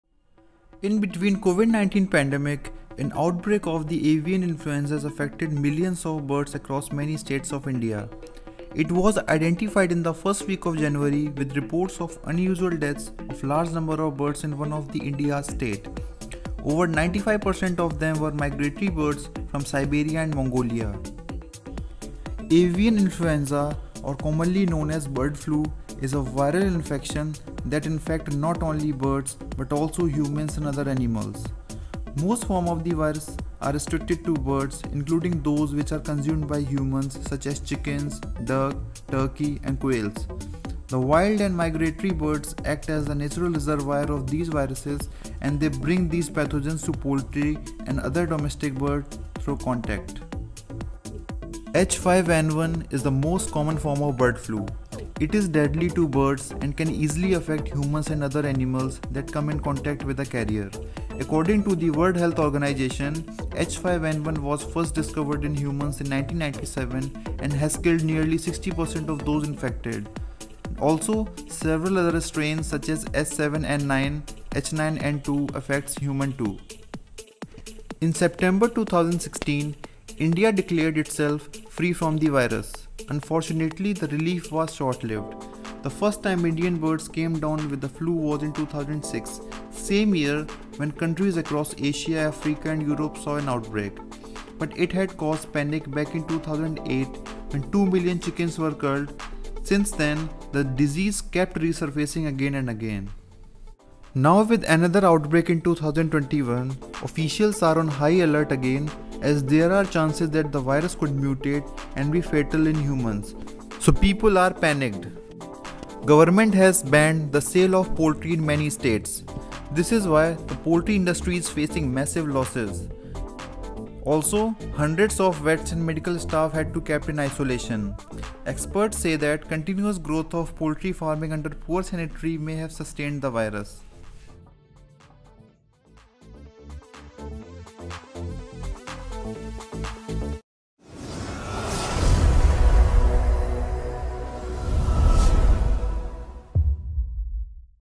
Rise of Bird Flu in India in 2021 | Should you worry about it? | H5N1 virus | Cursor
H5N1 is the most common form of bird flu.  It is deadly to birds and can easily affect humans and other animals that come in contact with a carrier. According to the
WHO , H5N1 was first discovered in humans in 1997 and has killed nearly 60 percent of those infected also Several other strains such as H7N9 , H9N2 affects human too.

In sept 2016 , India declared itself free from the virus, unfortunately the relief was short lived.  The first time Indian birds came down with the flu was in 2006 , same year when countries across Asia , Africa and Europe saw outbreak .But it  had caused panic  back in 2008 when 2 million chickens were culled since then the disease kept resurfacing again and again .